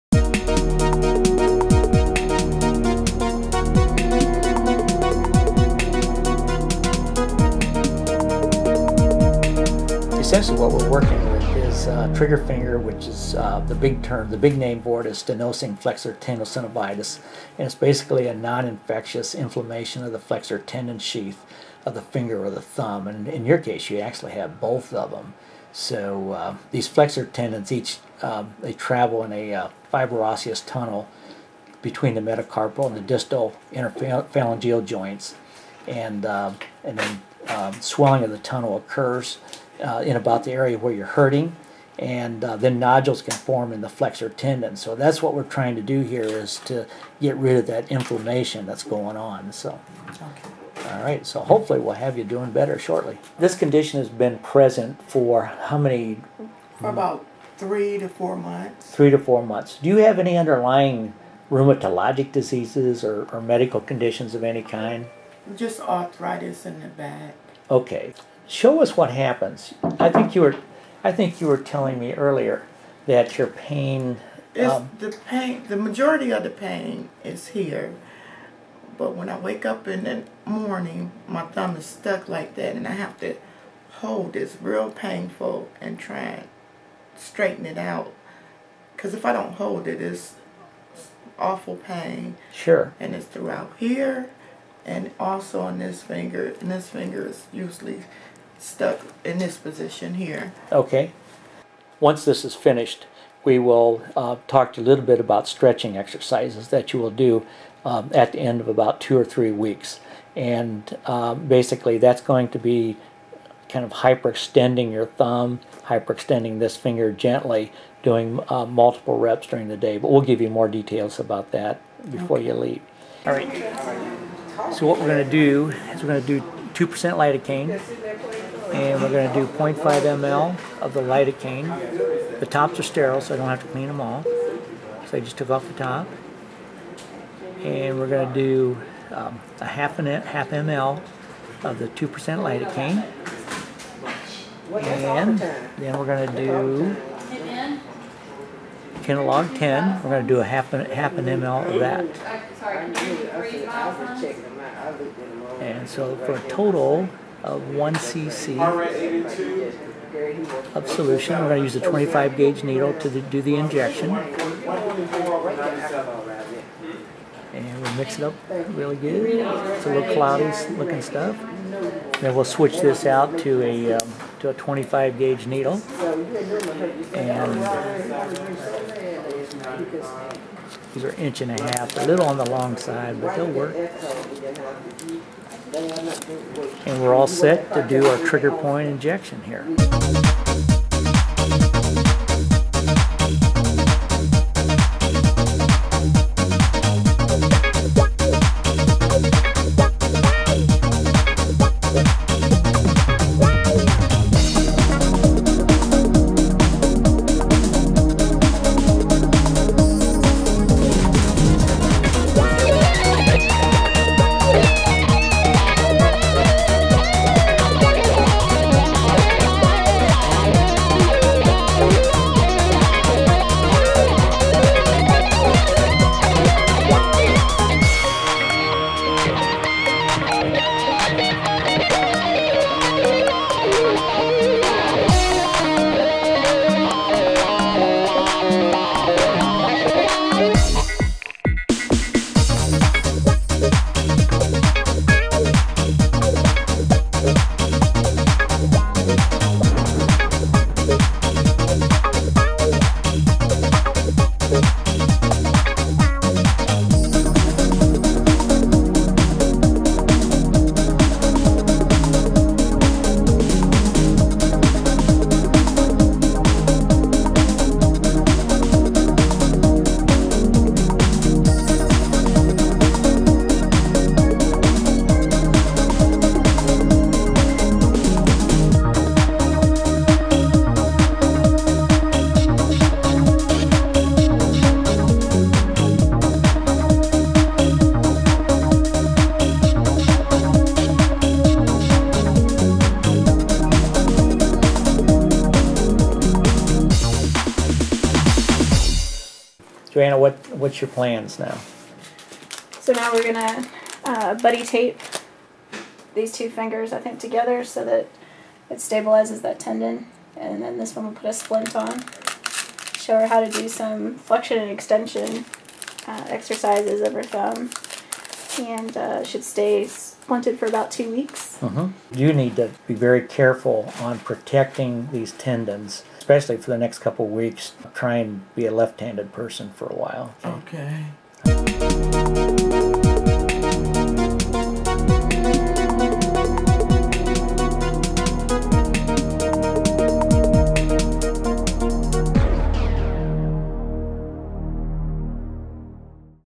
Trigger Finger Injection Therapy
In this video we demonstrate how to treat stenosing tenosynovitis or trigger finger by injecting steroid medication subcutaneously over the inflamed tendon.